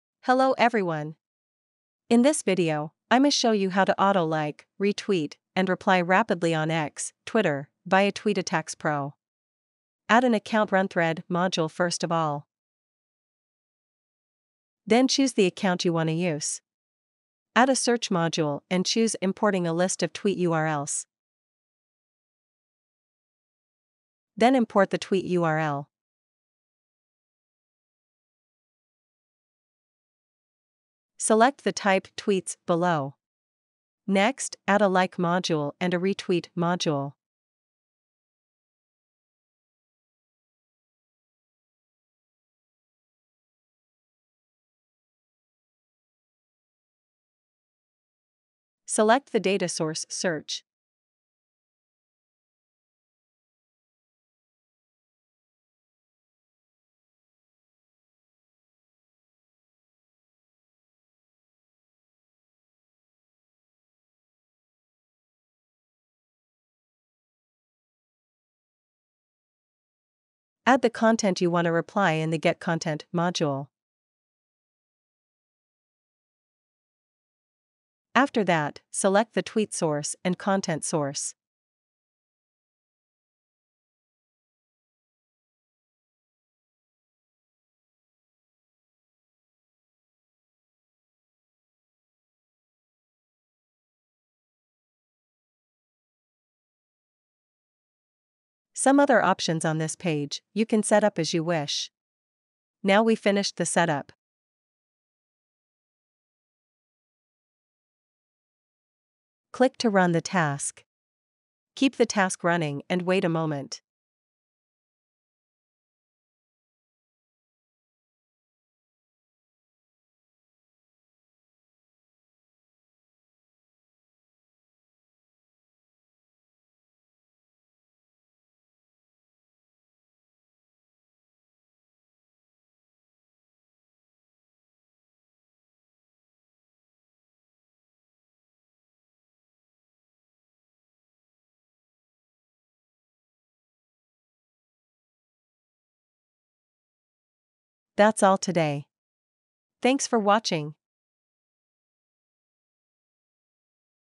Auto like, retweet, and reply tweets via TweetAttacksPro
This video shows you how to auto like, retweet, and reply rapidly on X (Twitter) via TweetAttacksPro.